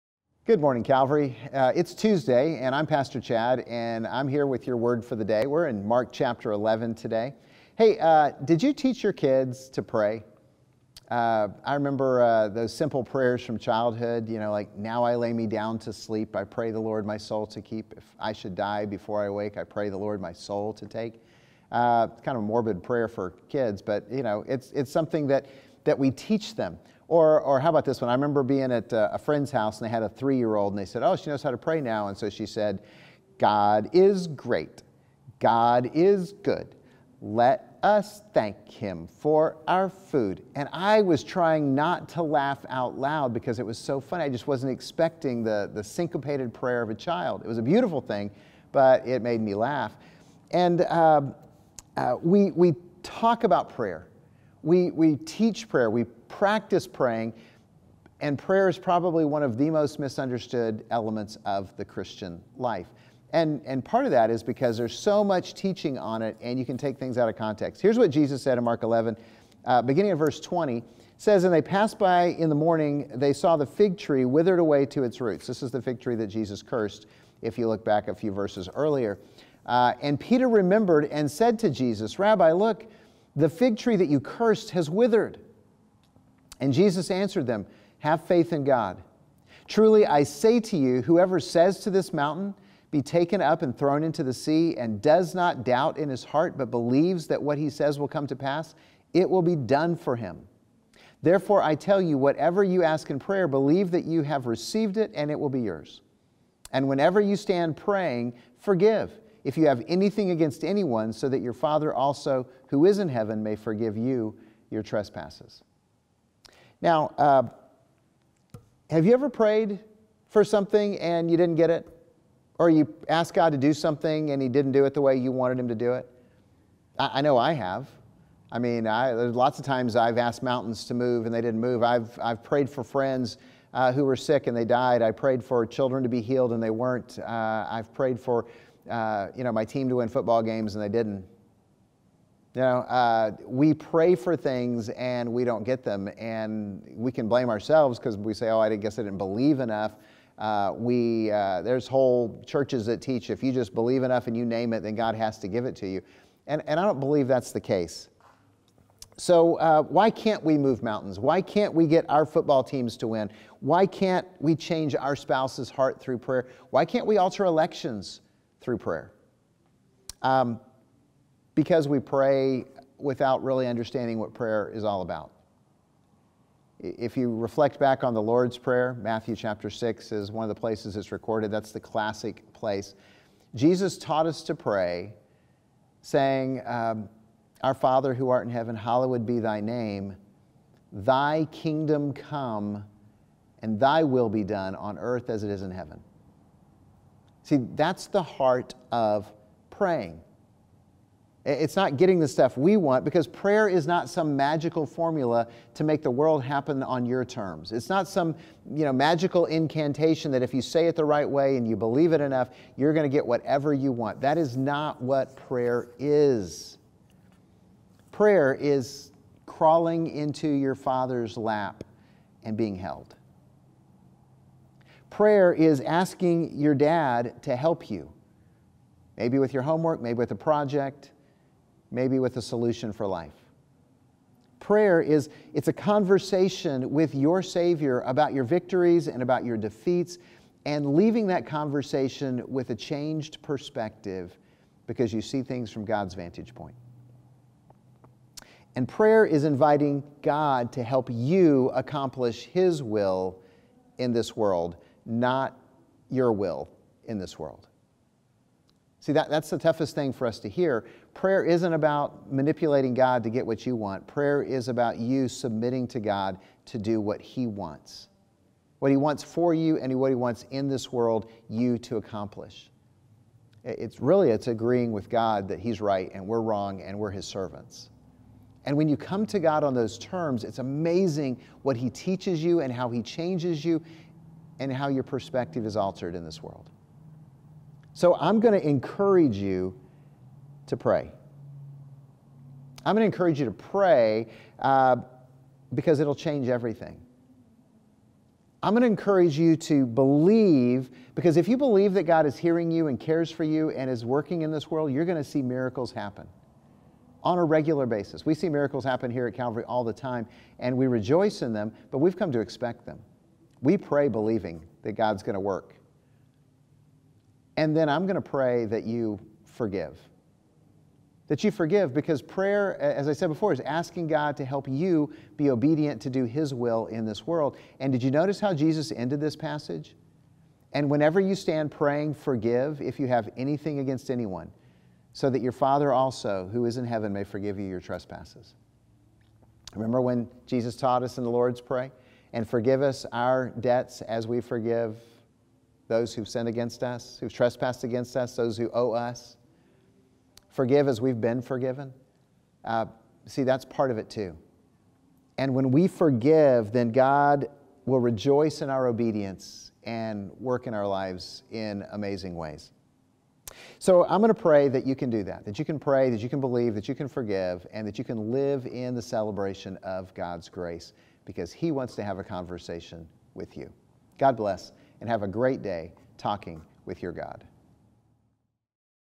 Your Word for the Day - Episode 176
On this Tuesday morning, Pastor Chad shares some helpful words on prayer and how we can grow closer to God through conversation with him.